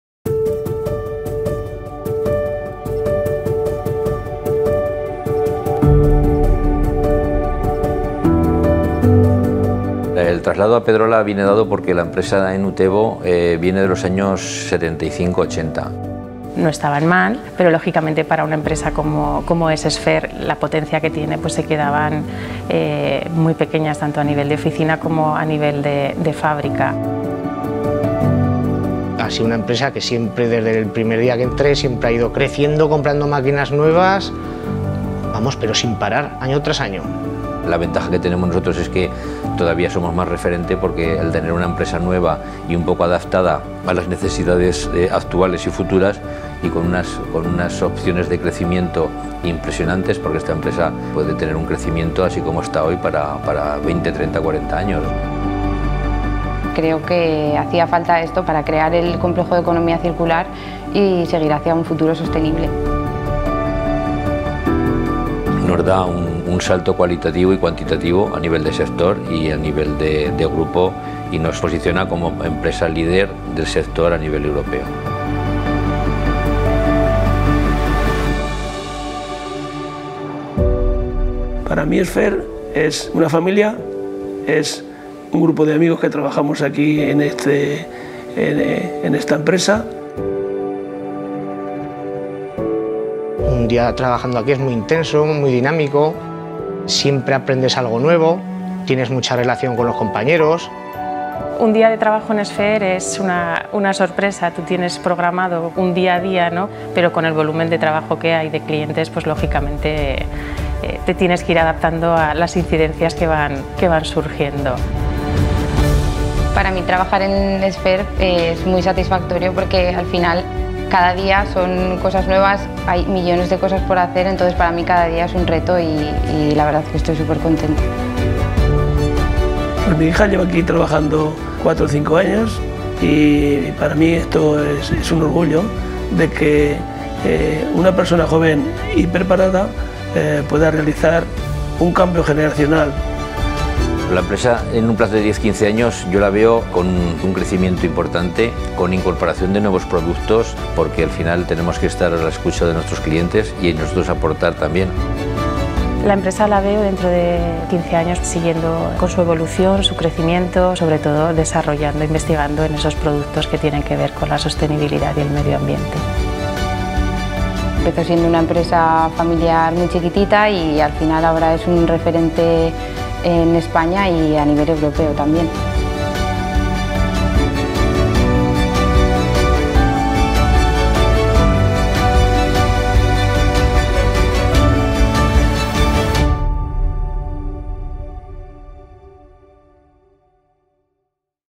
Video corporativo para la empresa Sphere Spain | DOSIS VIDEOMARKETING
Realización de un vídeo corporativo para la empresa española Sphere Spain dedicada a la fabricación de productos biodegradables.
En el vídeo se quería transmitir el valor que aporta a la empresa y a sus trabajadores el traslado a la nueva planta en Pedrola, en la que la economía circular y el crecimiento sostenible de la empresa es la base de su futuro.